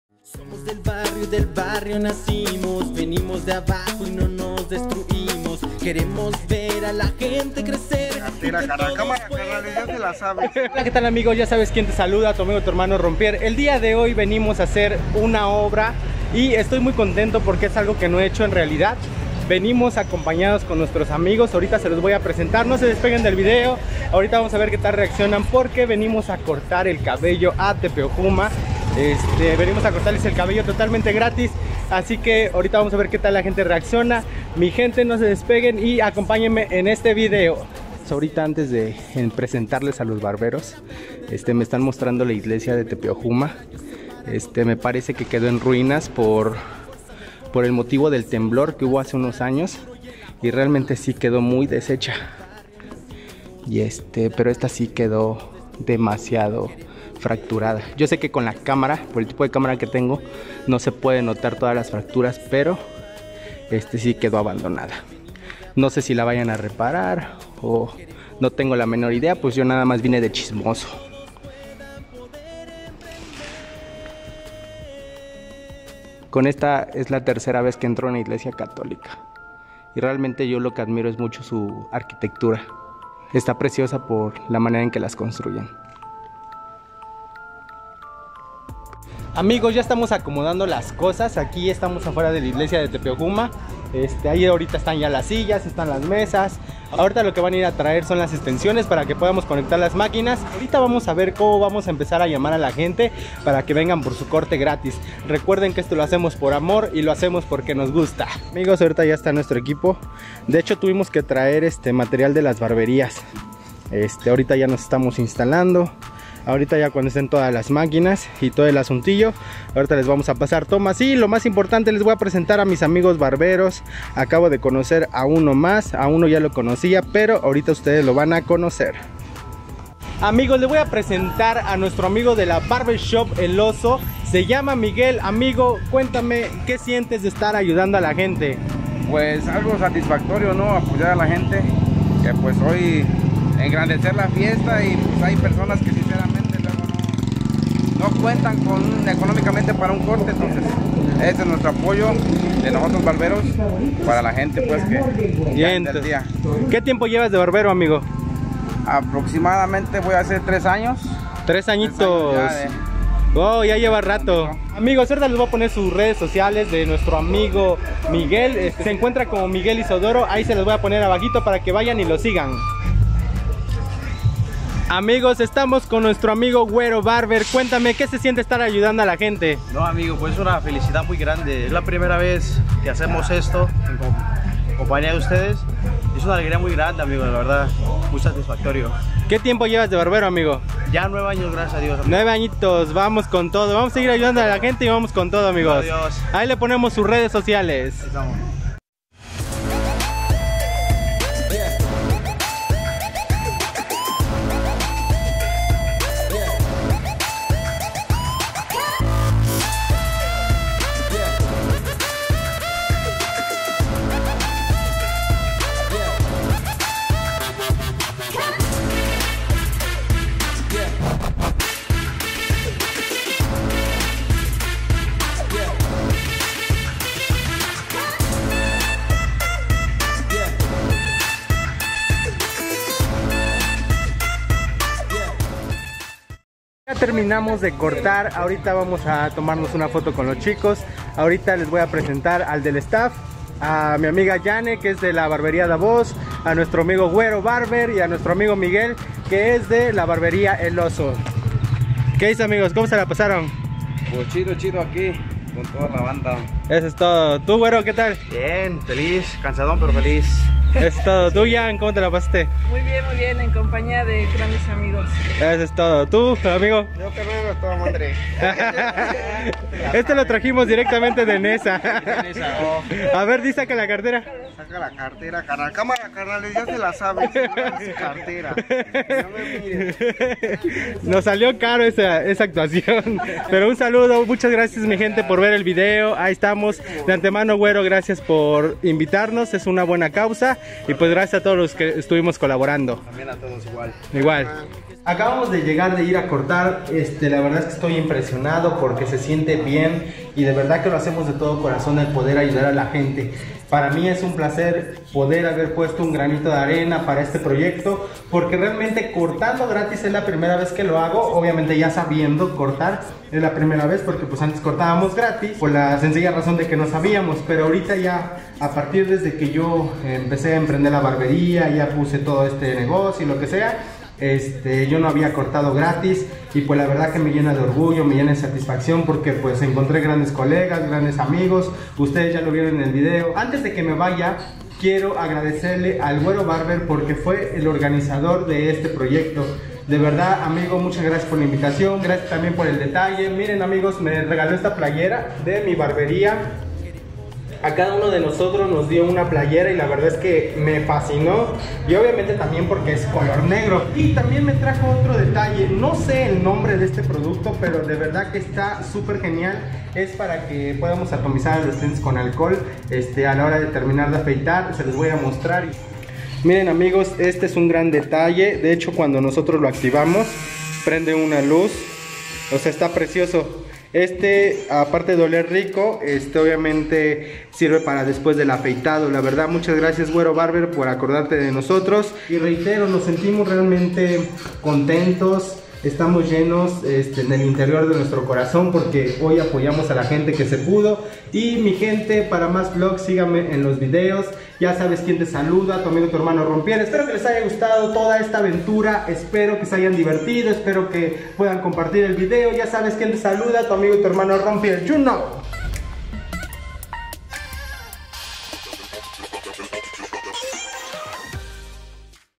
4 barberos regalando cortes de cabello - Acto benéfico - Rompier Vlogs
Familia está vez nos tocó apoyar a nuestra gente de  Tepeojuma que se encuentra en Atlixco Pue.
Regalamos cortes de cabello  para niños y personas de la tercera edad.

Si tienes conocimiento compártelo y si tienes la posibilidad de ayudar al que no tiene, hazlo!!

La vida se vive una sola vez